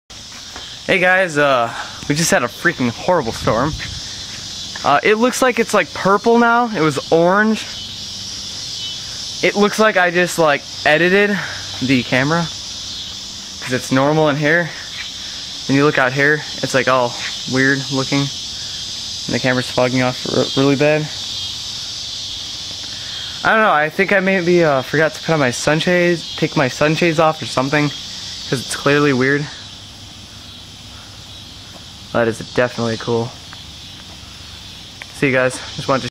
Orange Skys After A Horrible Storm
Outside after a brutal storm.